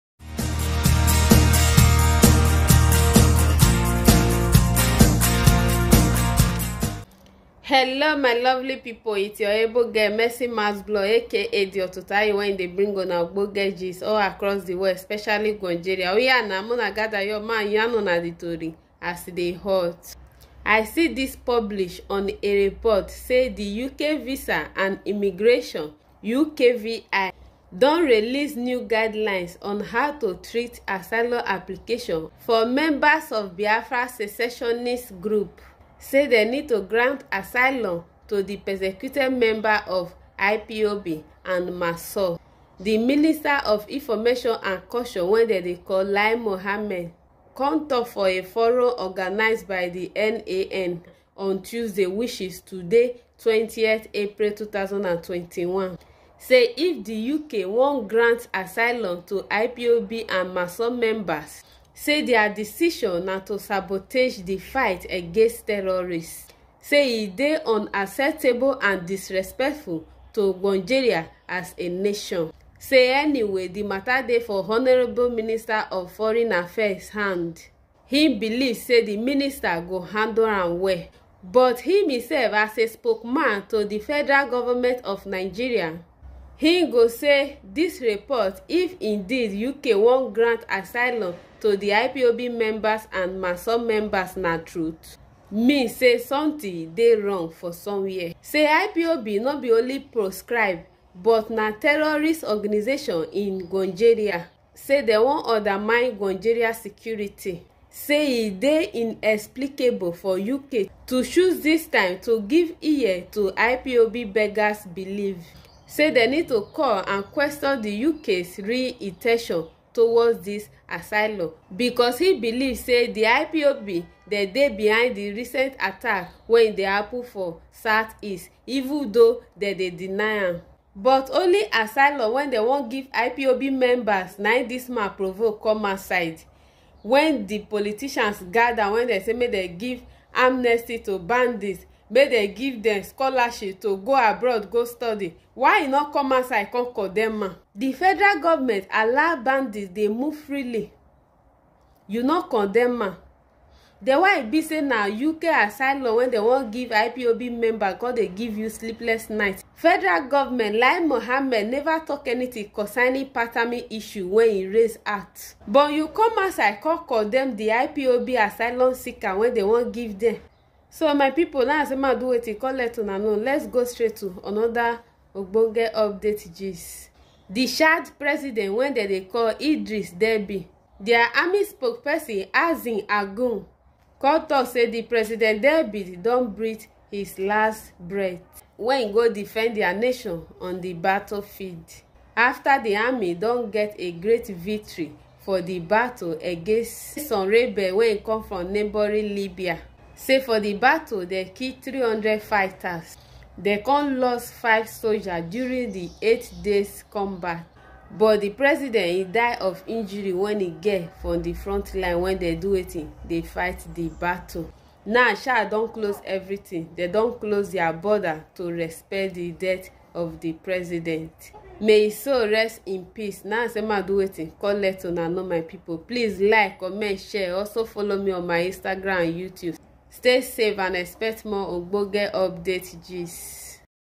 FG Tackles UK On Plan To Grant IPOB Members Asylum.
FG Tackles UK On Plan To Grant IPOB Members Asylum    / Chad's President Derby Dies Of Injuries Suffered On Front line Battle. (This page does not support violence) 20th April 2021.